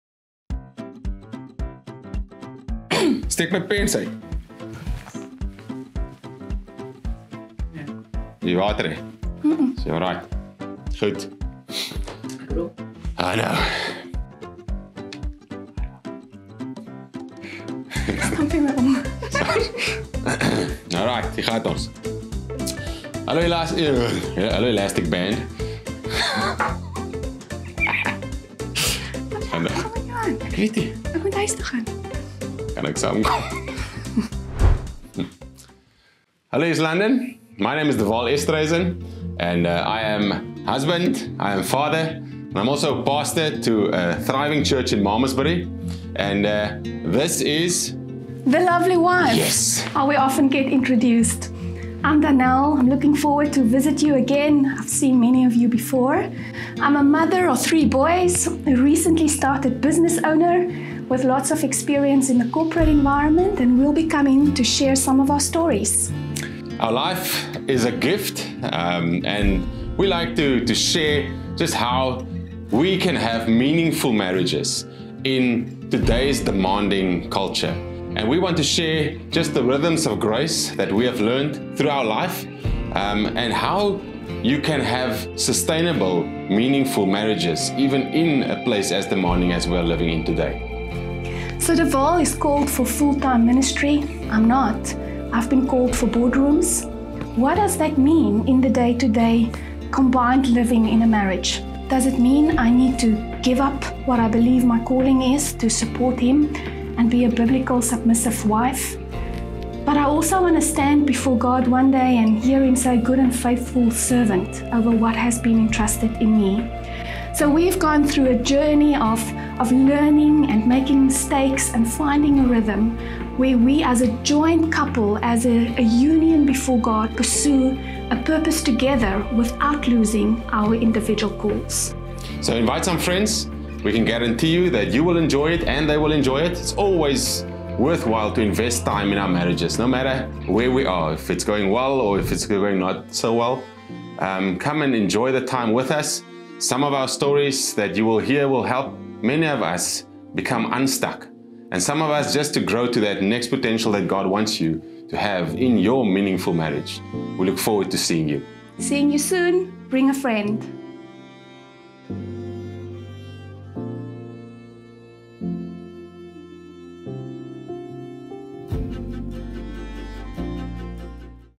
Marriage Enrichment Weekend with De Waal & Danelle Esterhuizen
Breathe new life into your marriage! 💕

Join us at the marriage enrichment weekend with De Waal and Danelle Esterhuizen.
14 - 15 July 2023 at Shofar East London.

for more info about Shofar East London and directions to church.